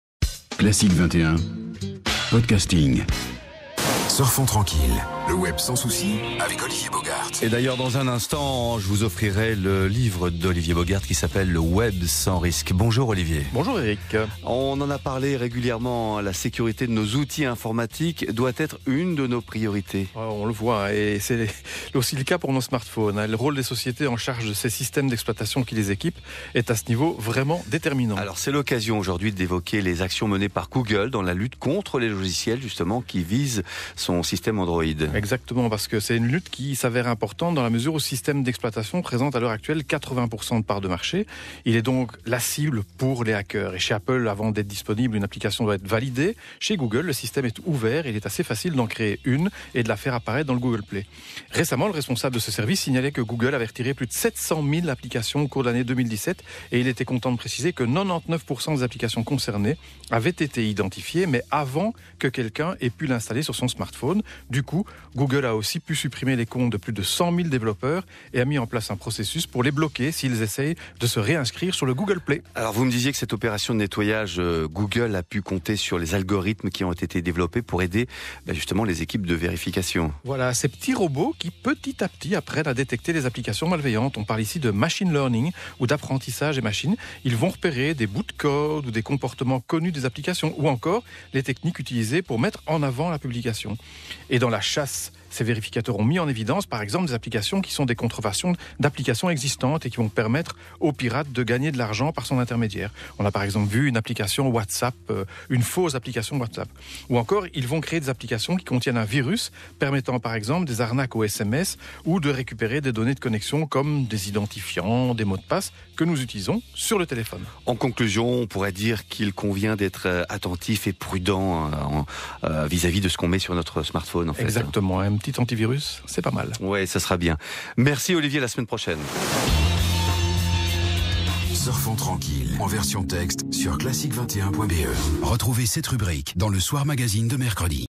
Surfons Tranquille: la sécurité de nos outils informatiques
Nous en parlons régulièrement, la sécurité de nos outils informatiques doit être une de nos priorités. C’est donc aussi le cas pour nos smartphones et le rôle des sociétés, en charge des systèmes d’exploitation qui les équipent, est à ce niveau déterminant. L’occasion d’évoquer les actions menées par Google dans la lutte contre les logiciels qui visent son système Android.

Une lutte d’autant plus importante que ce système d’exploitation représente à l’heure actuelle 80 % de parts de marché. Il est donc une cible de choix pour les hackers. En effet, alors que chez Apple, avant d’être disponible, une application doit être validée, chez Google, le système est ouvert : il est assez facile d’en créer une et de la faire apparaître dans le Google Play.

Récemment, le responsable de ce service signalait que Google avait retiré plus de 700.000 applications au cours de l’année 2017 et il était content de préciser que 99% des applications concernées avaient été identifiées avant que quelqu’un ait pu l’installer sur son smartphone.

Du coup, Google a aussi pu supprimer les comptes de plus de 100.000 développeurs et a mis en place un processus pour les bloquer s’ils essayent de se réinscrire sur le Google Play. Dans cette opération de nettoyage, Google a pu compter sur les algorithmes qui ont été développés pour aider les équipes de vérification.

Des robots qui, petit à petit, apprennent à détecter les applications malveillantes. On parle ici de " machine learning " ou d’apprentissage des machines. Ils vont repérer des bouts de codes ou des comportements connus des applications. Ou encore, les techniques utilisées pour mettre en avant la publication.

Et, dans leur chasse, ces vérificateurs ont mis en évidence des applications qui sont des contrefaçons d’applications existantes et qui vont permettre aux pirates de gagner de l’argent pas son intermédiaire. Ou encore, des applications qui contiennent un virus permettant, par exemple, des arnaques au SMS ou de récupérer des données de connexion comme des identifiants et des mots de passe que nous utilisons sur notre téléphone.

Si donc, le retrait de toutes ces applications est une bonne nouvelle, nous devons quand même rester attentifs et prudents avec ce que nous installons sur notre smartphone.